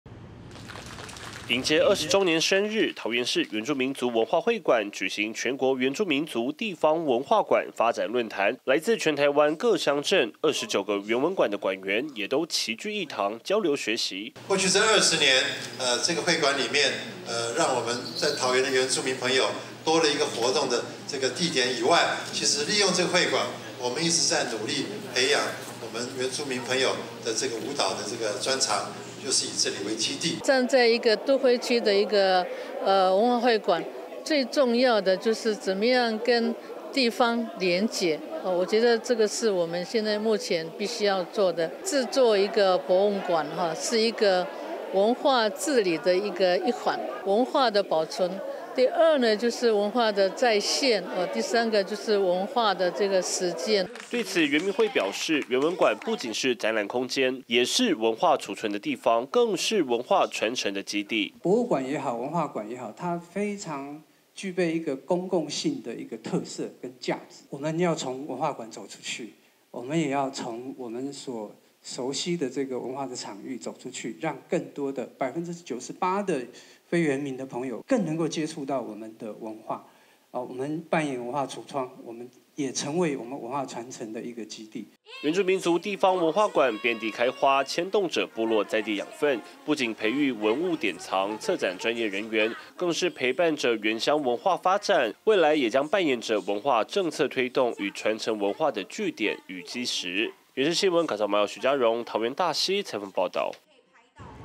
迎接桃原館20年 辦全國原文館發展論壇｜每日熱點新聞｜原住民族電視台
桃園市原住民族文化會館 20歲生日了，13日這天舉辦了 全國原住民族地方文化館發展論壇，全台29個原文館館員齊聚一堂，相互交流學習，探究接下來原文館在地方發展的願景與期盼。